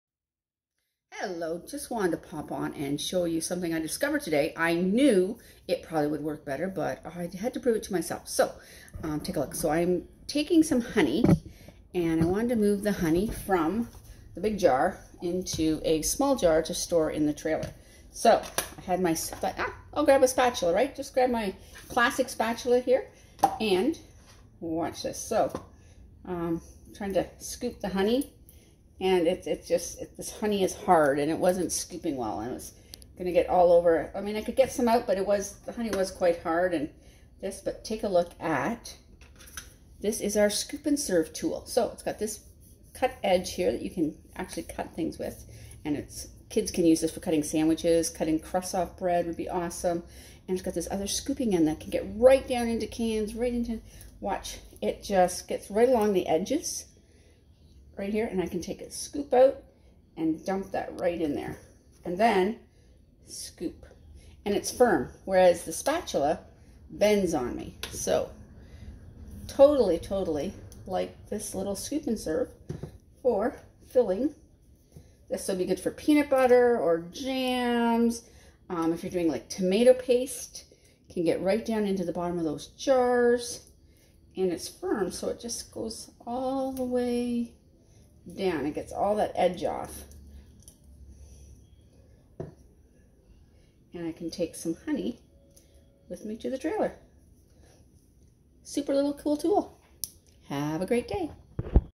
Pampered Chef Scoop and Serve
This little tool is often overlooked but is fantastic at scraping out all those cans of tomato paste or getting the honey out of the jar! The other end has a serrated edge perfect for sandwiches.